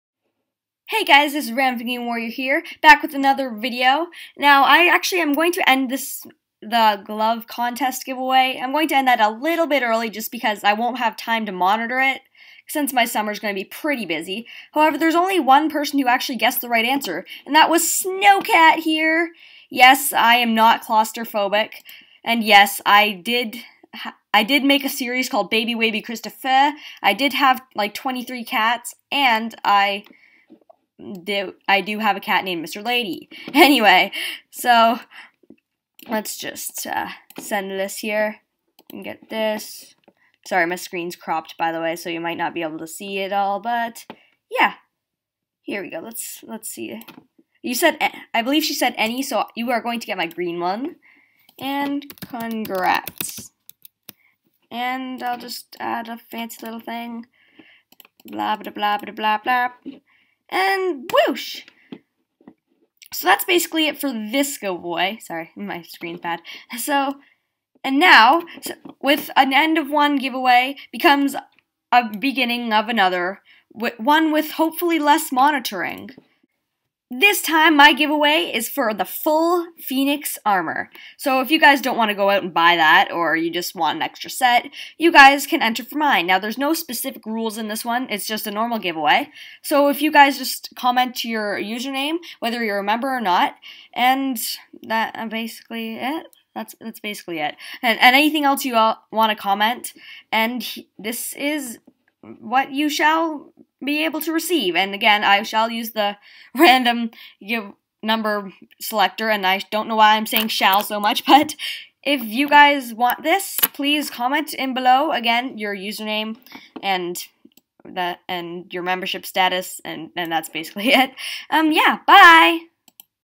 FULL PHEONIX ARMOR GIVAWAY + Glove Givaway Winner (Read Description)
I had to end my glove give-away early because due to my schedule, i couldn't monitor it anymore. Hope you had fun with it!

Time:  Right Now -- Sept. 15
Rules:
-You can only comment ONCE or else you will be disqualified.
-Comment your Username and Membership status

Have Fun, and sorry for the early end to the last give-away!